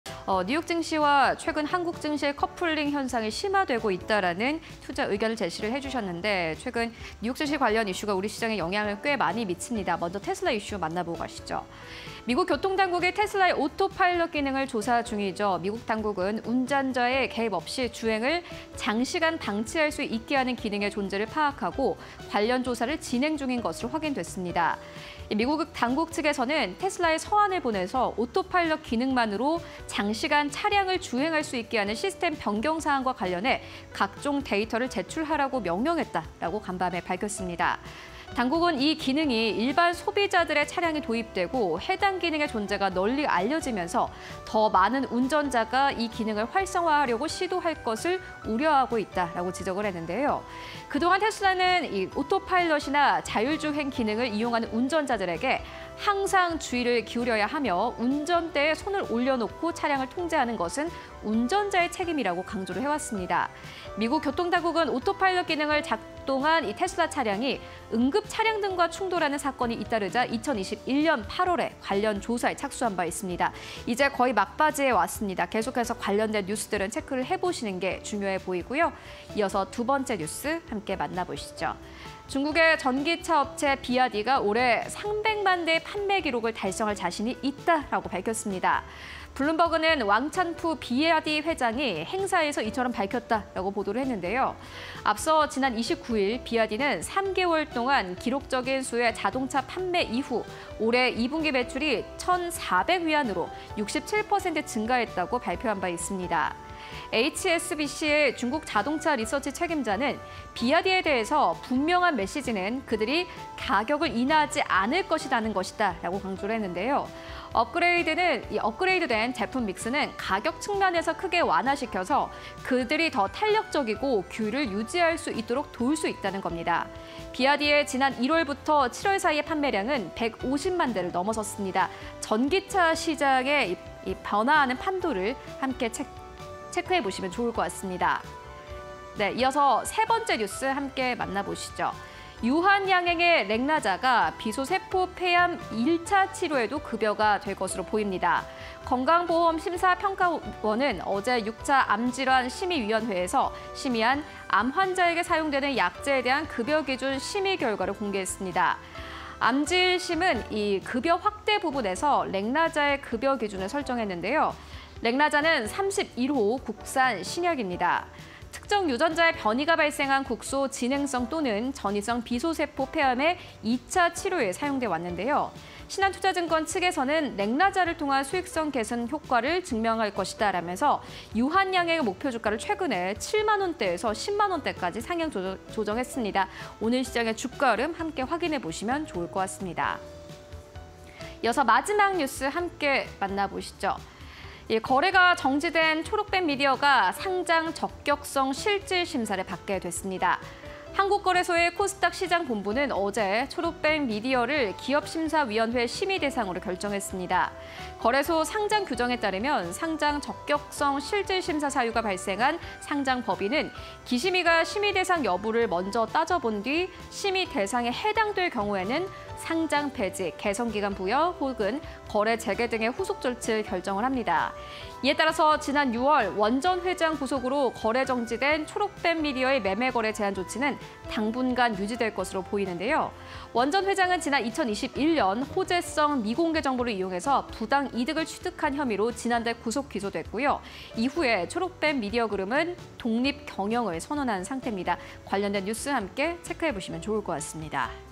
美당국 “테슬라 장시간 방치 가능한 기능 조사 중” / BYD "올해 300만대 판매 달성 확신" / 유한양행 '렉라자', 건강보험 첫 관문 통과_굿모닝 VIEW (20230831
# 美 당국 “테슬라 장시간 방치 가능한 기능 조사 중”
- 테슬라, 오토파일럿 켜고 장시간 방치 가능
- 미 당국 “장시간 방치 기능이 운전자 부주의 늘려”
- 테슬라 “차량 통제는 운전자의 책임”

BYD “올해 300만대 판매 달성 확신”
- 왕찬푸 회장, 행사 참여해 확신 발언
- 2분기 매출, 1400억 위안 기록… 전년 대비 67% 증가
- HSBC “가격 인하하지 않는다는 메시지”
- 1~7월 판매량, 150만대 넘어

유한양행 '렉라자', 건강보험 첫 관문 통과
- 건심평, 국산 폐암약 렉라자 급여기준 설정
- 렉라자, 국내서 31번째로 개발된 표적 항암제
- 비급여로 약값 1년에 7천만원 수준의 ‘고가’
- 앞으로 1차 치료에도 급여될 것으로 전망

초록뱀미디어 상장 적격성 실질심사 대상 지정
- 코스닥시장본부, 초록뱀미디어 기심위 심의 대상 결정
- 원영식 전 회장 배임 혐의로 사유 발생
- 매매거래 제한 조치 당분간 유지